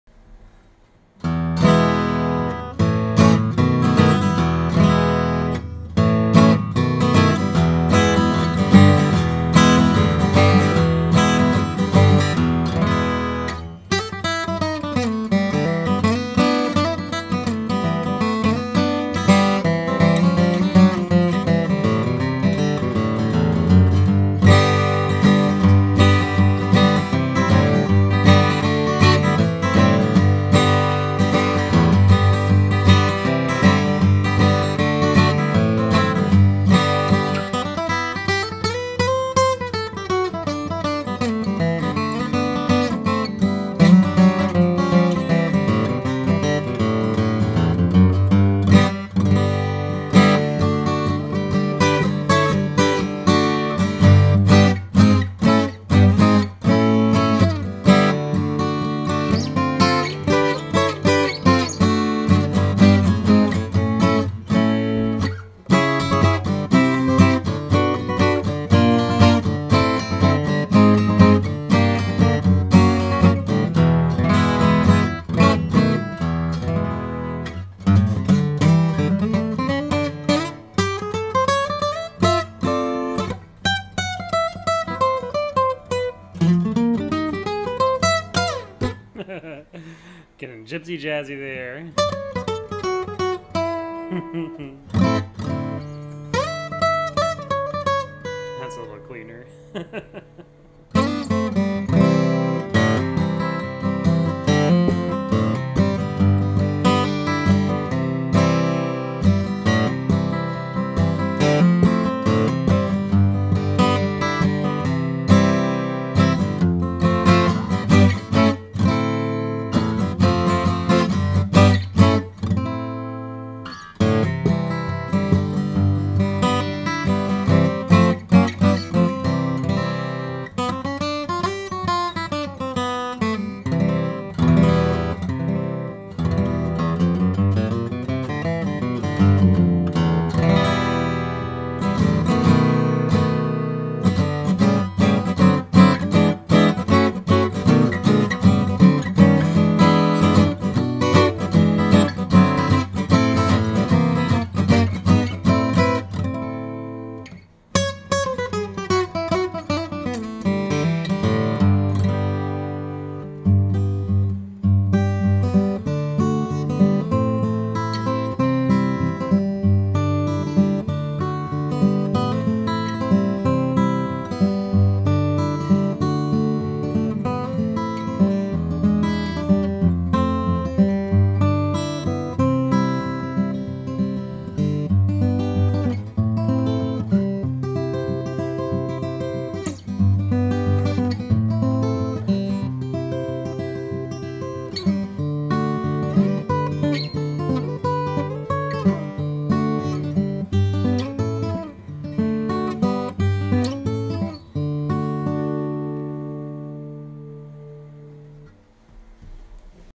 1971 Guild D-40 dreadnought guitar
I’ve got two Guild D-40s here at the moment. This is the 1971 model and was built at the (then-new) Westerly, RI factory.

It’s still very much in the vein of the old Hoboken D-40s and sounds and plays a lot like one, though it’s got the drop-in-saddle bridge and a little bit more of that “upper-mids punch” that I associate with the company’s ‘70s dreadnoughts.

These are great alternatives to D-18s and punch-out forward beautifully. I gave this one a neck reset, recut the saddle slot and made a new saddle, gave it a fret level/dress, removed a “Bridge Doctor” someone had installed, and set it all up. It’s a fast player, for sure.